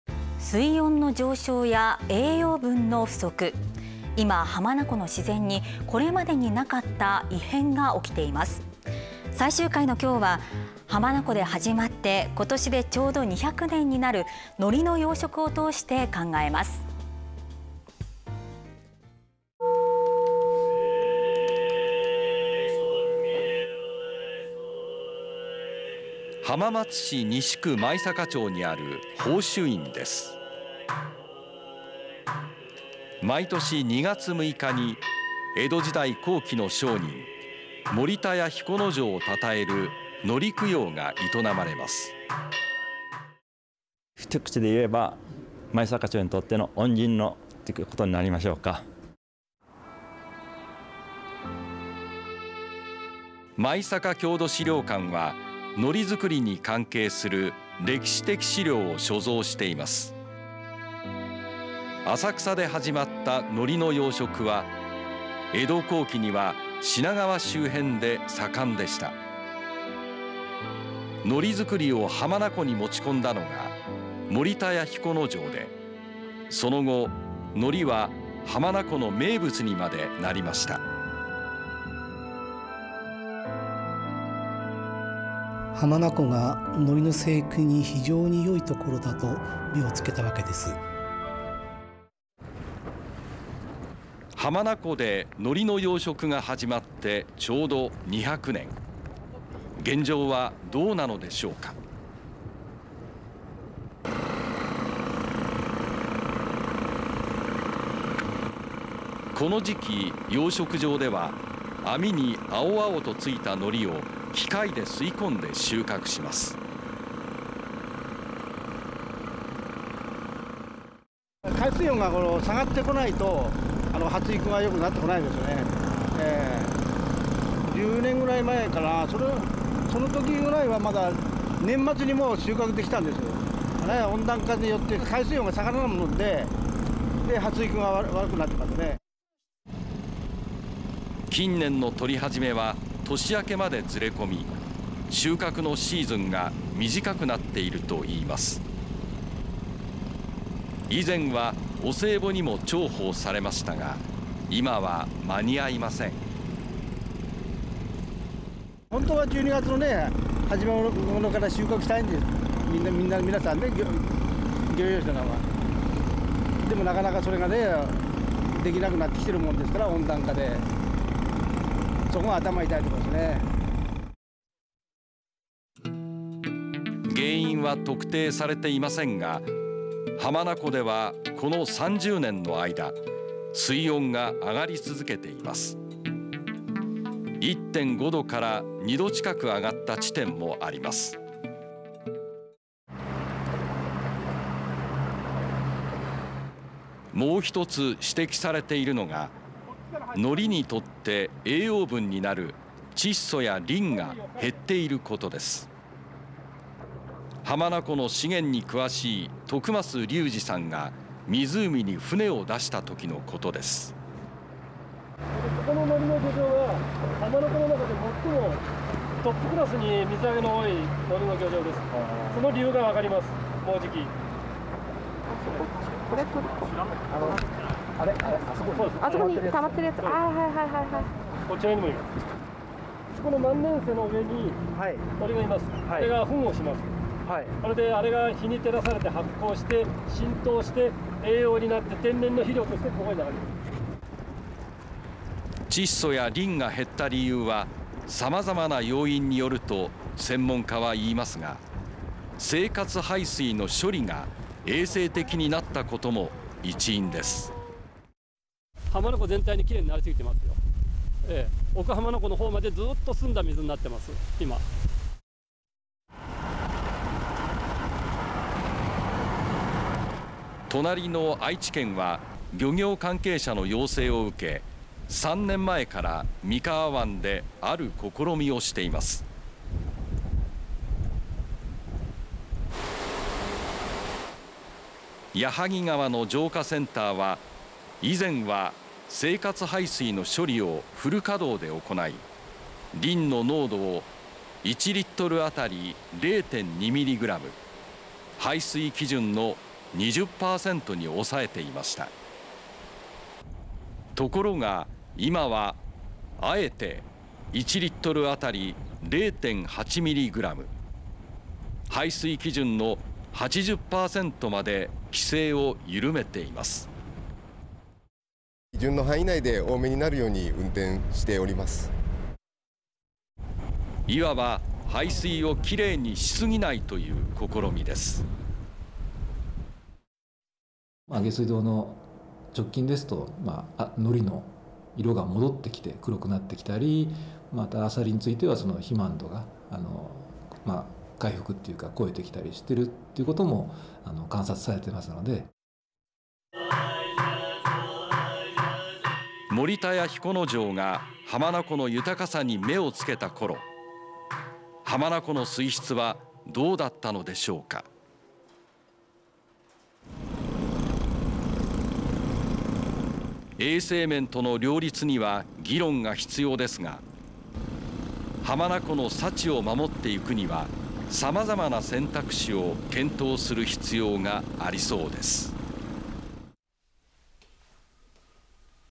浜名湖の異変③２０２０年で２００年 ノリの養殖は
水温の上昇や栄養分の不足。いま浜名湖の自然にこれまでになかった異変が起きています。最終回は浜名湖で始まって２０２０年でちょうど２００年になるノリの養殖を通して考えます。
　浜松市西区舞阪町にある宝珠院です。毎年２月６日に、江戸時代後期の商人、森田屋彦之丞を称える海苔供養が営まれます。
　片桐三之住職「一口で言えば舞阪町にとっての恩人」
　舞阪郷土資料館はノリづくりに関係する歴史的史料を所蔵しています。浅草で始まったノリの養殖は江戸後期には品川周辺が大産地でした。ノリづくりを浜名湖に持ち込んだのが森田屋彦之丞で、その後、ノリは浜名湖の名物にまでなりました。
　舞阪郷土資料館　荒熊元茂館長「浜名湖がノリの生育に非常に良い所だと目をつけたわけです」
　浜名湖でノリの養殖が始まってちょうど２００年。現状はどうなのでしょうか。この時期、養殖場では網に青々と付いたノリを機械で吸い込んで収穫します。
　ノリ養殖業者　白柳達夫さん「水温が下がってこないと発育がよくなってこない。１０年ぐらい前は年末に収穫できたんです。温暖化で水温が下がらないので発育が悪くなっている」
　近年の採り始めは年明けまでずれ込み、収穫のシーズンが短くなっているといいます。以前はお歳暮にも重宝されましたが、今は間に合いません。
　ノリ養殖業者　白柳達夫さん「本当は１２月の初めから収穫したい、漁業者は。でもそれができなくなってきている、温暖化で。そこが頭が痛いところ」
　原因は特定されていませんが、浜名湖ではこの３０年の間、水温が上がり続けています。１．５度から２度近く上がった地点もあります。
　もう一つ指摘されているのが、ノリにとって栄養分になる窒素やリンが減っていることです。浜名湖の資源に詳しい徳増隆二さんが湖に船を出した時のことです。
　浜名漁協　徳増隆二さん「ここのノリの漁場は浜名湖でもトップクラスの水揚げ。その理由がわかります、もうじき」
　浜名漁協　徳増隆二さん「瀬の上に鳥がいます。フンをします。日に照らされて発酵して栄養になって天然の肥料になります」
　窒素やリンが減った理由は、様々な要因によると専門家はいいますが、生活排水の処理が衛生的になったことも一因です。
　浜名漁協　徳増隆二さん「浜名湖全体できれいになりすぎている。奥浜名湖までずっと澄んだ水」
　隣の愛知県は、漁業関係者の要請を受け３年前から三河湾である試みをしています。矢作川の浄化センターは、以前は生活排水の処理をフル稼働で行い、リンの濃度を１リットル当たり０．２ミリグラム、排水基準の２０％に抑えていました。ところが今は、敢えて１リットル当たり０．８ミリグラム、排水基準の８０％まで規制を緩めています。
　愛知県西三河建設事務所　橋本尚人さん「基準の範囲内で多めになるように運転しています」
　いわば排水をきれいにしすぎないという試みです。
　愛知県水産試験場漁場環境研究部　蒲原聡部長「（放流口の）直近ですとノリの色が戻ってきて黒くなってきたり、アサリは肥満度が回復していることが観察されている」
　森田屋彦之丞が浜名湖の豊かさに目を付けたころ、浜名湖の水質はどうだったのでしょうか。衛生面との両立には議論が必要ですが、浜名湖の幸を守っていくには様々な選択肢を検討する必要がありそうです。